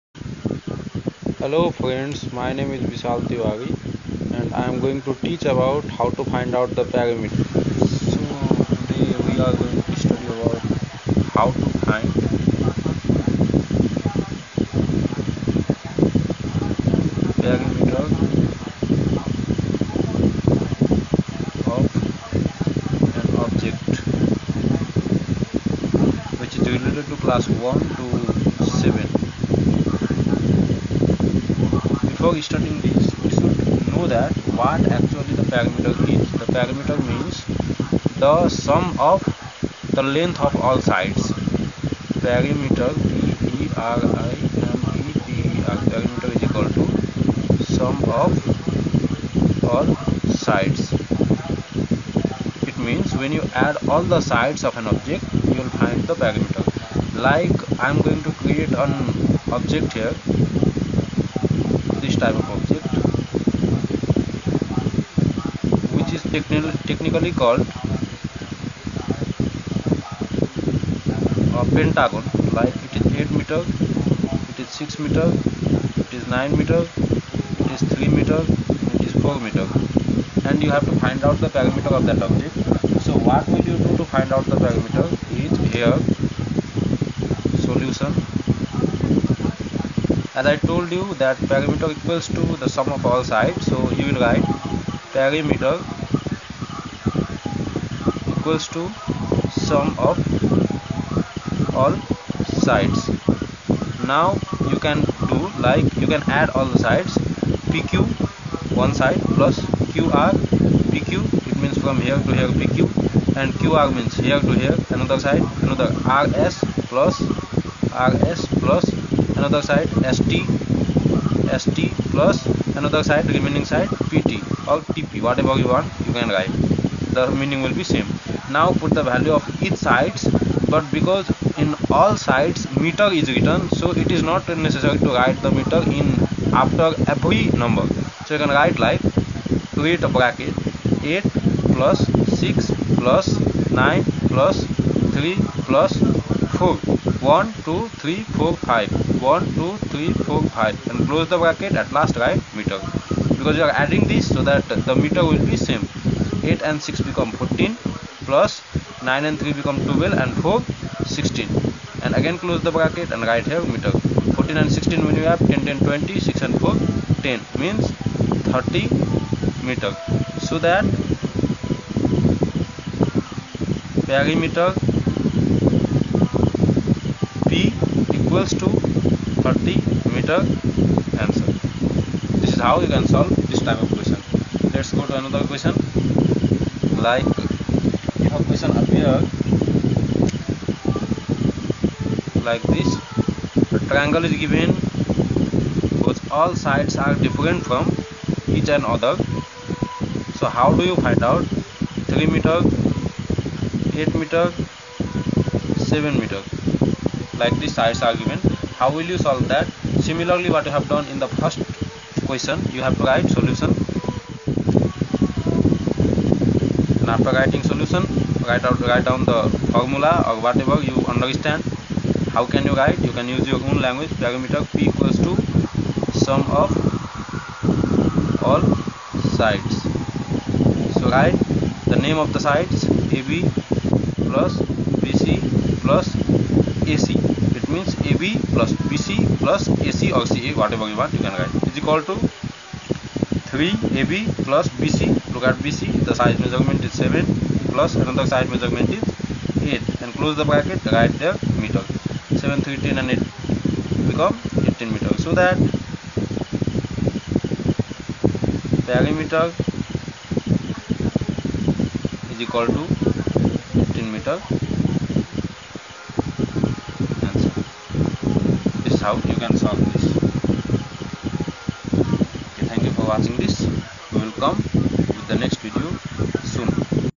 How to find perimeter of any shape, By Bishal Sir
Hello friends its my first try so the quality of sound and picture are not as they have to be ...I will come back with more confident and quality videos very soon